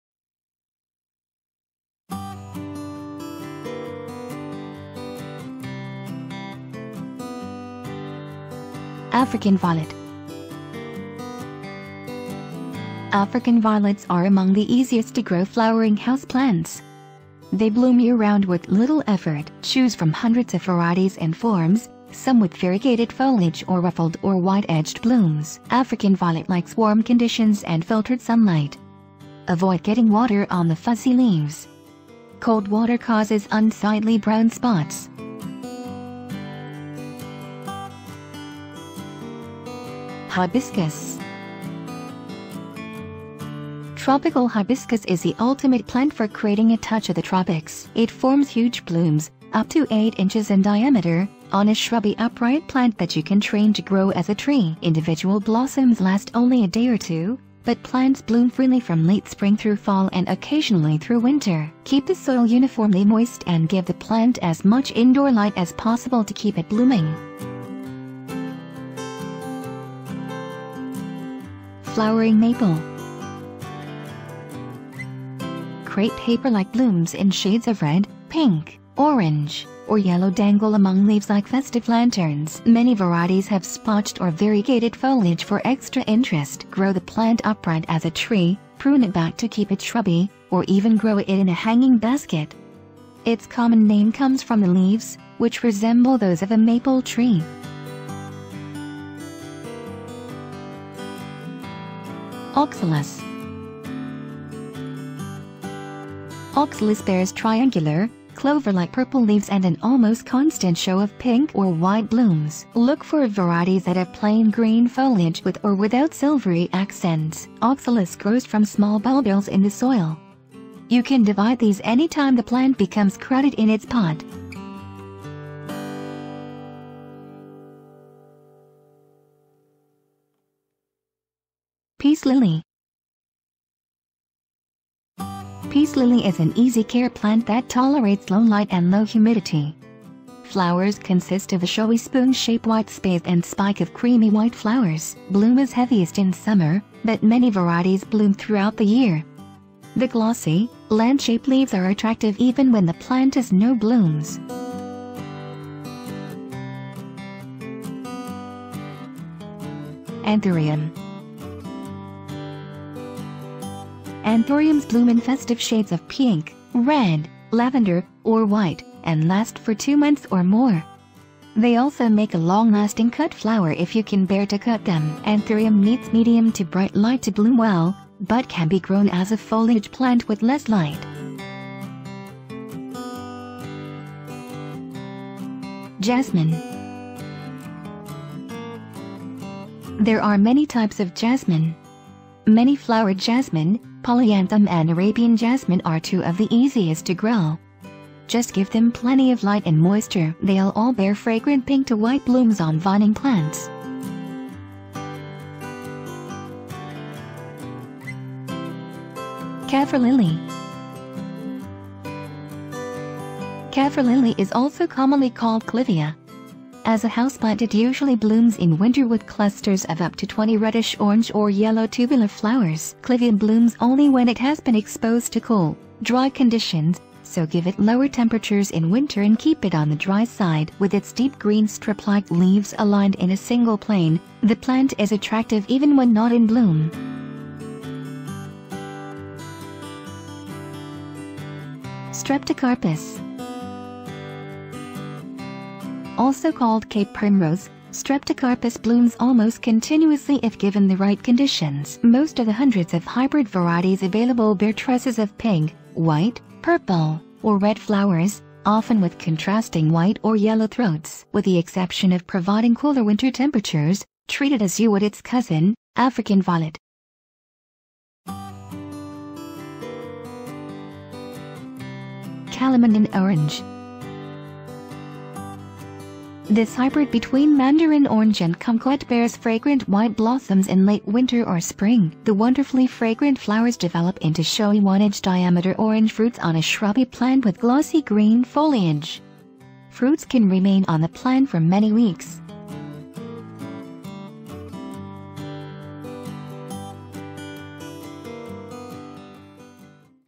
Top 10 Beautiful Blooming Houseplants
Home furniture store Just6F constantly updated beautiful designs with great price.
Find fragrance and beauty in flowering houseplants. The blooming beauties described here will help you pick the best ones for your home.

1. African Violet
2. Hibiscus
3. Flowering Maple
4. Oxalis
5. Peace Lily
6. Anthurium
7. Jasmine
8. Kaffir Lily
9. Streptocarpus
10. Calamondin Orange